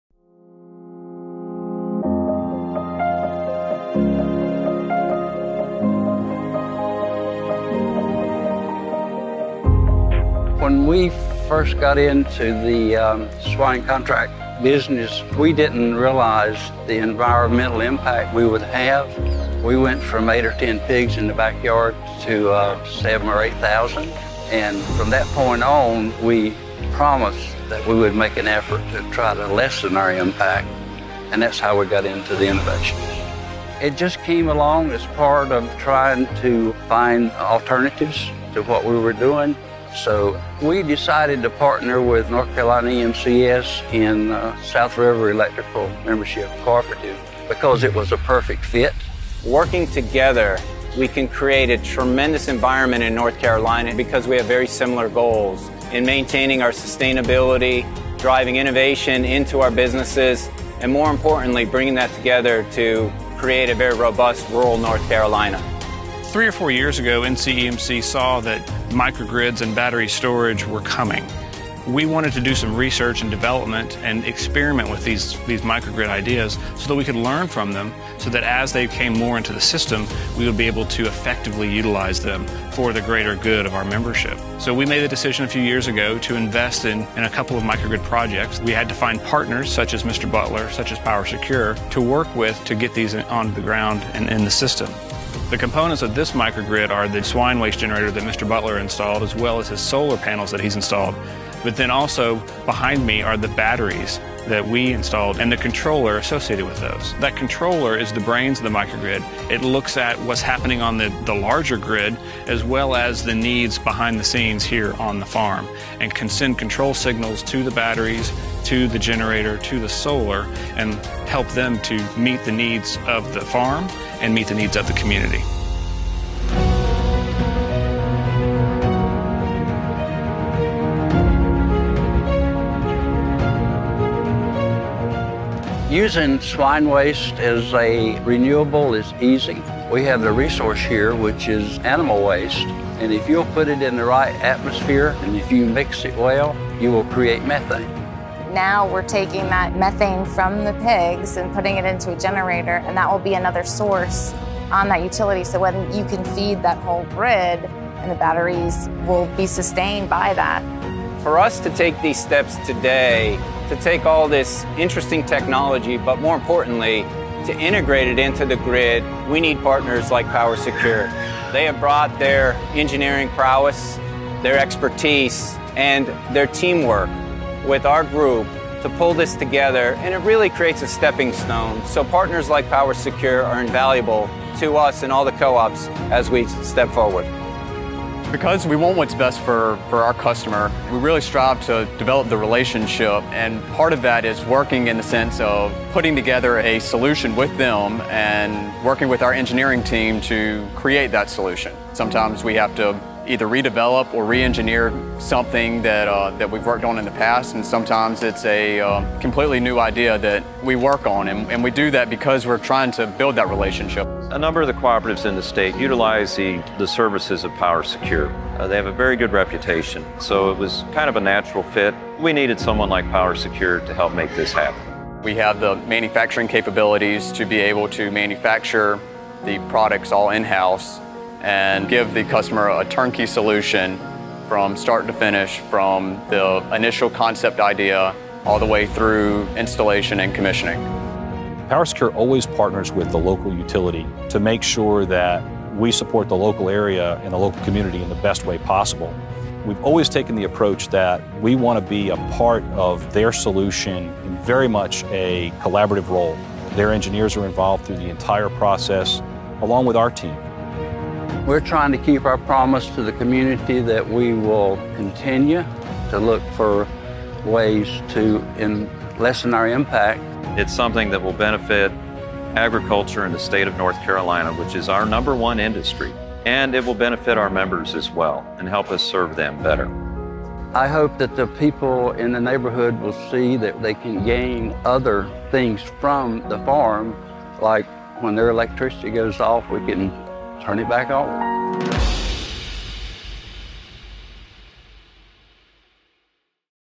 Butler Farms
PowerSecure partners with N.C. Electric Cooperatives. South River EMC and Butler Farms to design, build and engineer a one-of-a-kind biogas microgrid.